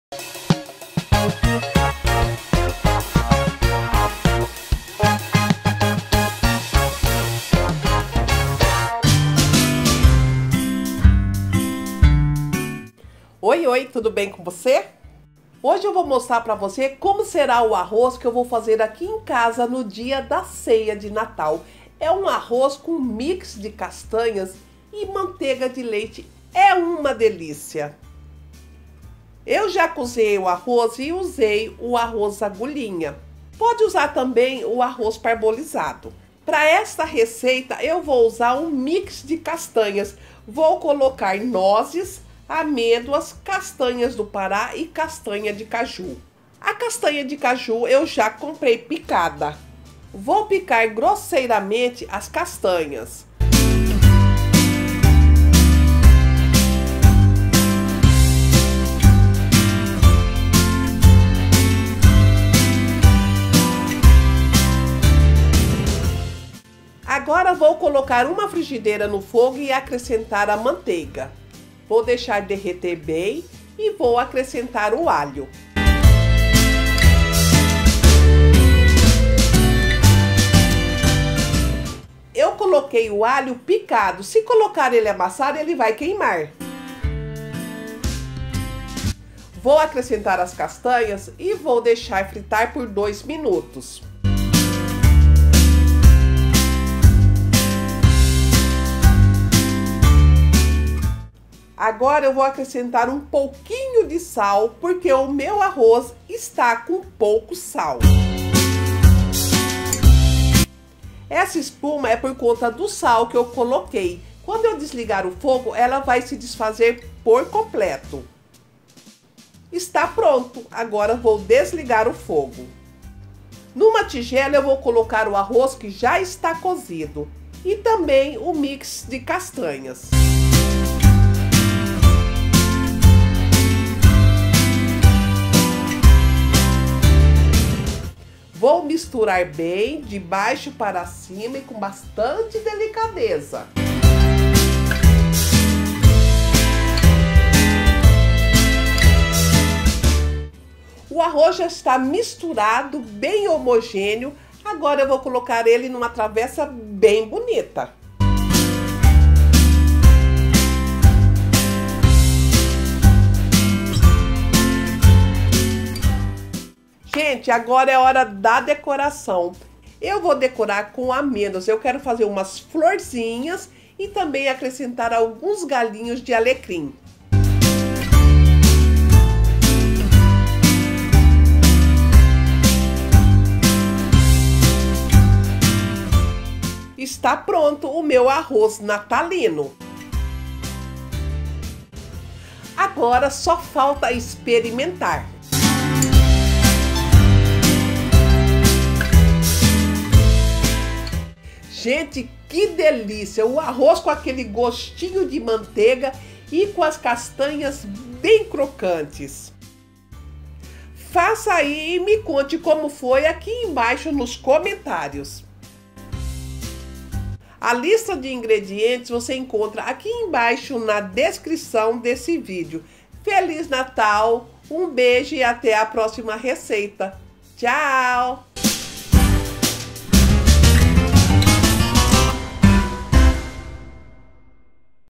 O MELHOR ARROZ NATALINO COM MIX DE CASTANHAS - FÁCIL E RÁPIDO DE FAZER.
moqueca vegana páscoa arrozcomcastanhas arroznatalino facildefazer receitadenatal ceiadenatal arrozcomcastanhas Receita deliciosa para a sua Ceia de Natal! Você está procurando uma receita de Arroz para a sua Ceia de Natal?
Já encontrou. Essa é a receita ideal para você. Arroz com Mix de Castanhas, delicioso, crocante e fácil de fazer.

2 copos de arroz (cru)
50 gr. de nozes
50 gr. de amêndoas
50 gr. de catanha do Pará
50 gr. de castanha de caju
3 colhers de manteiga com sal

Para decorar

Amêndoas, alecrim e flor Kalanchoe.